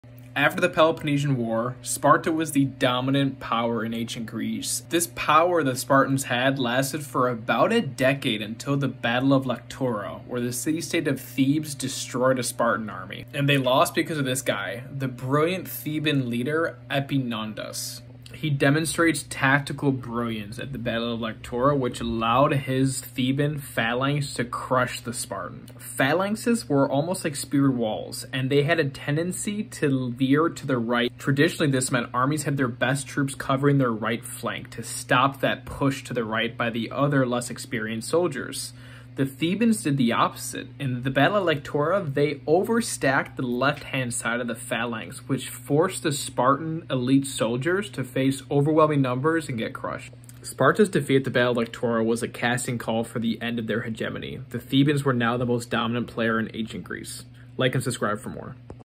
The Battle that crushed Sparta
The Battle of Lectura ended Spartan dominance in Ancient Greece, so why does no one learn about it? I'm not sure, but here is an all-together too brief version of events.